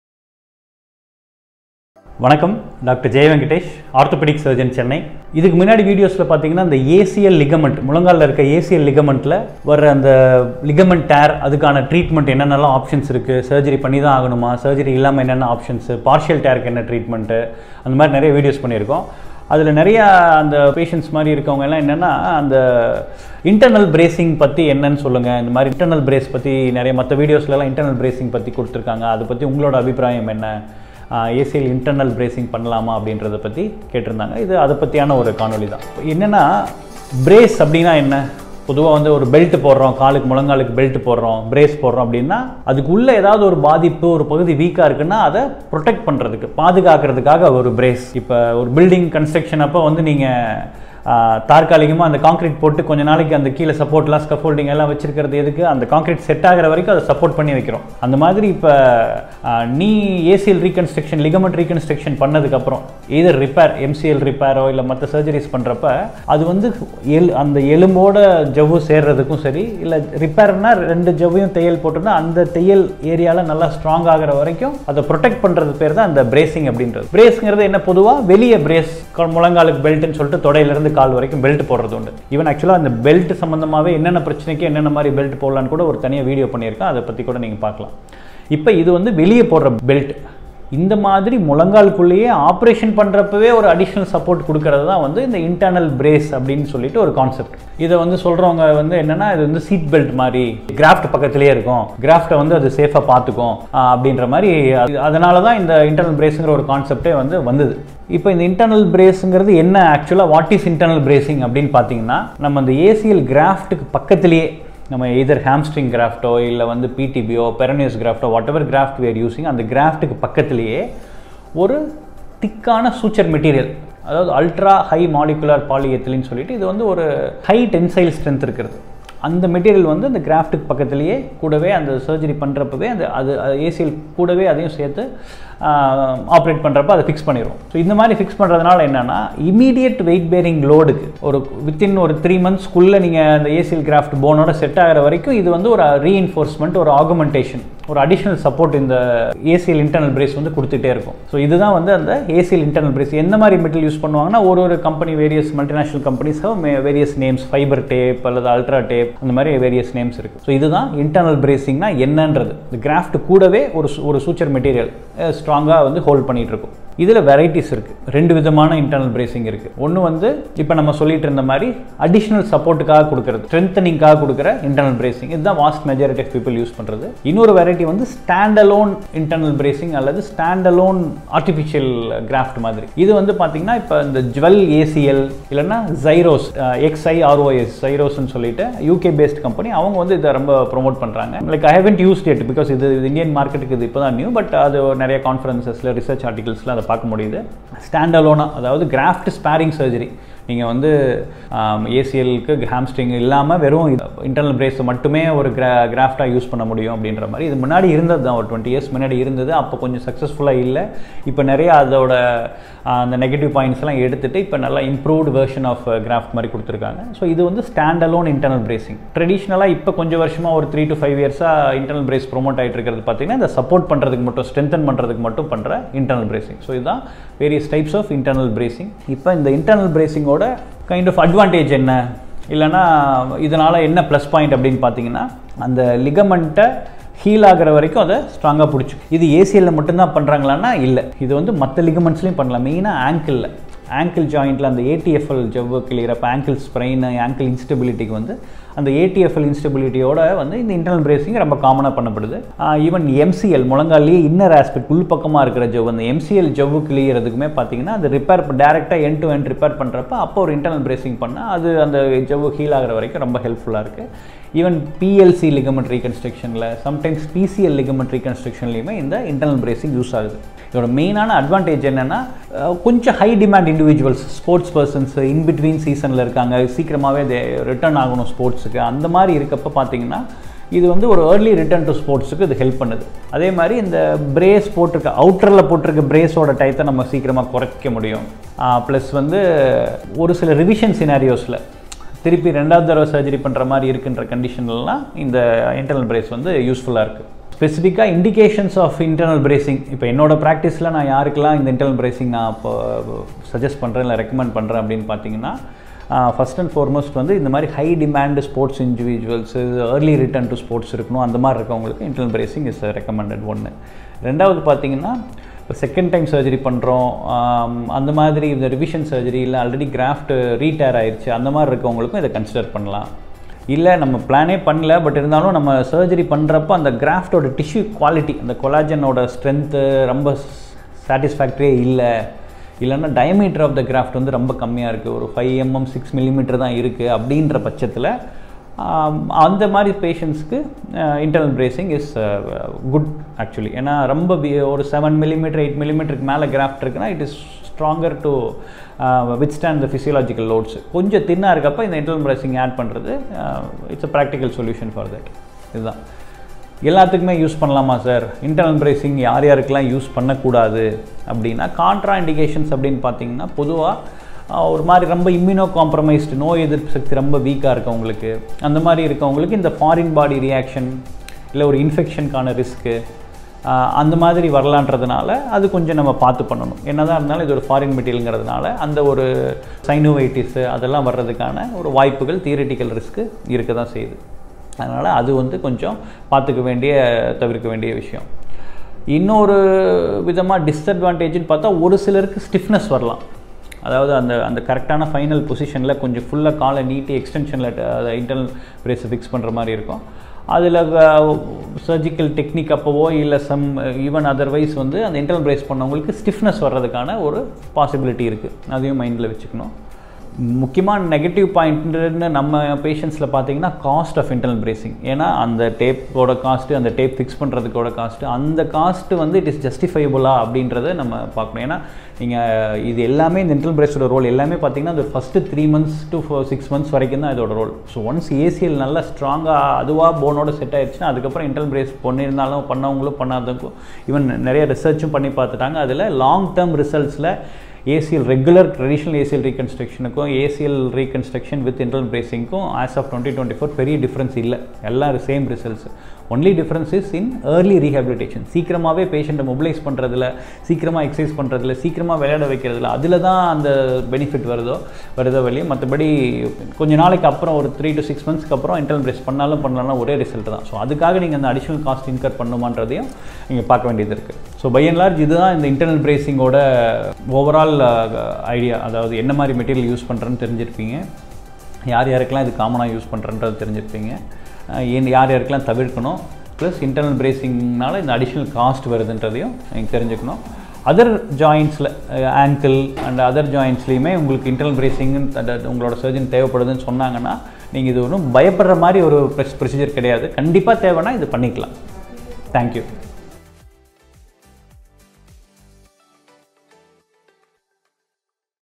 ACL Internal Brace Surgery - Explanation | Dr Jeya Venkatesh
Welcome to our in-depth guide on Internal Brace Surgery! In this video, we break down everything you need to know about this innovative procedure, which is revolutionizing the way we treat ligament and tendon injuries.